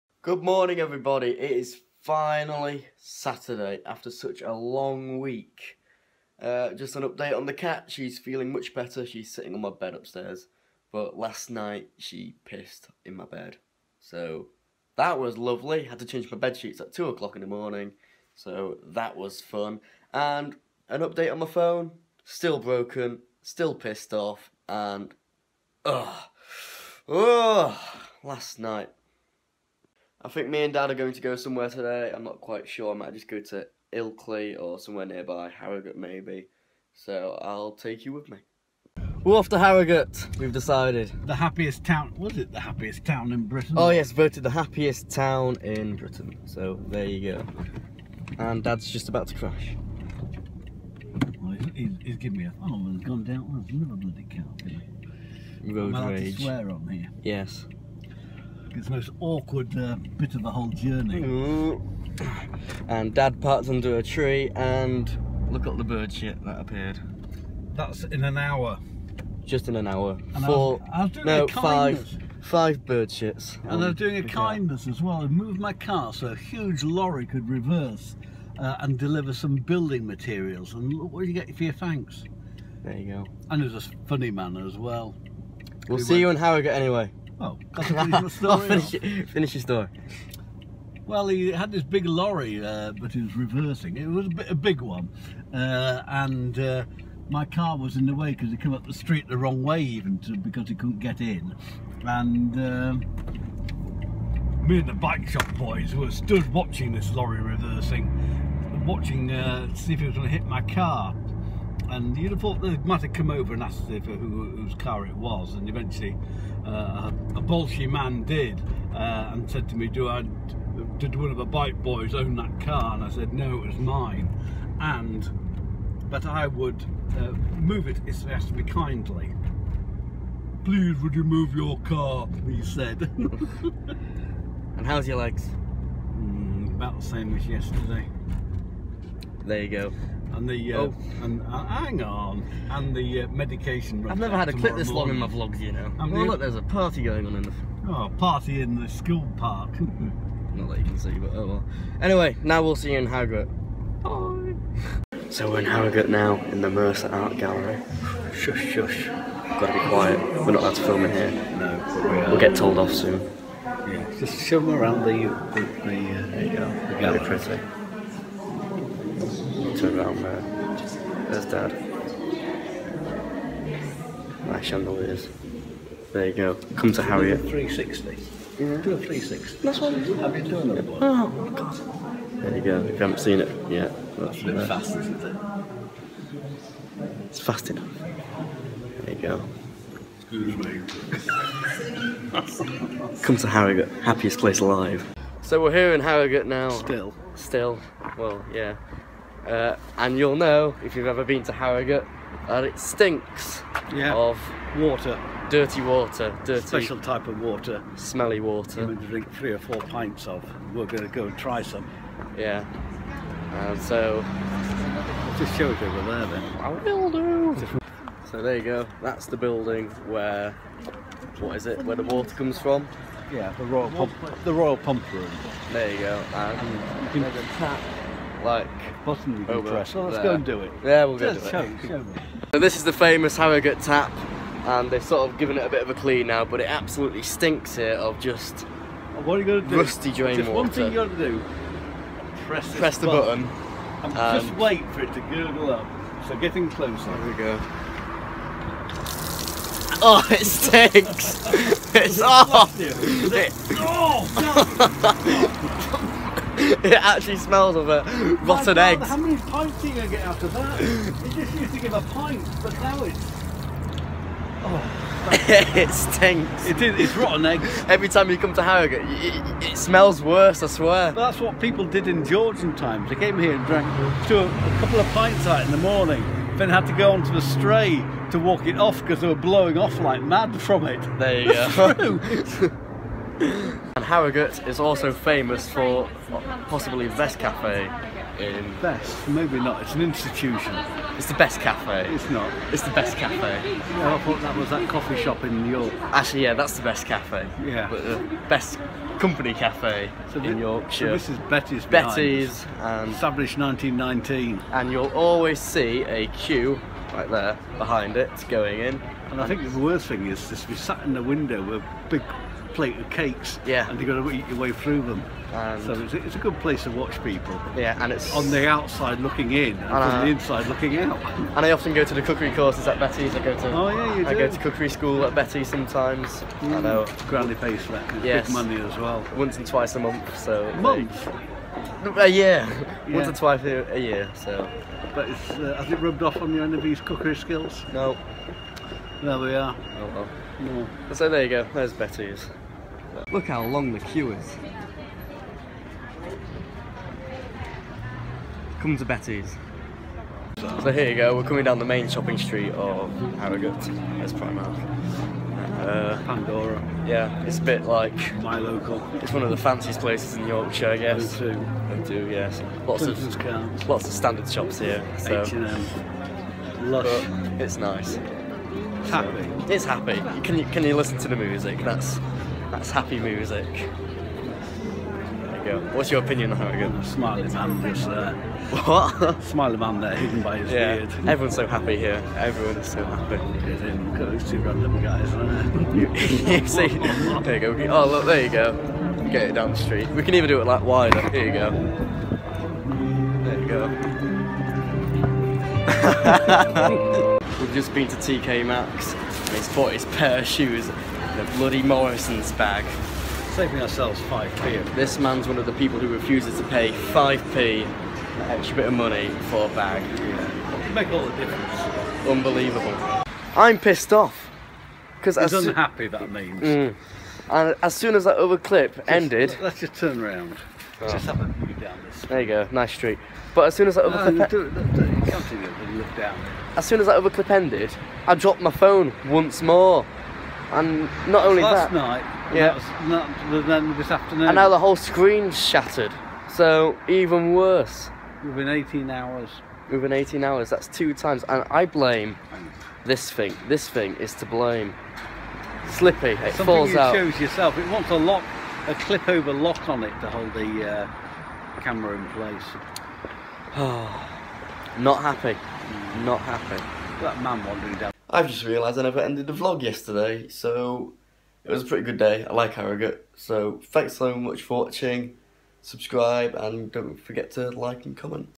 I broke my phone again!
Vlog: 009

Today we visited Harrogate which features one of the best cafe's in Yorkshire, Betty's go there! My experience was shortly ruined afterwards when I dropped my phone again! Twice in 2 days! I hate the selfie stick!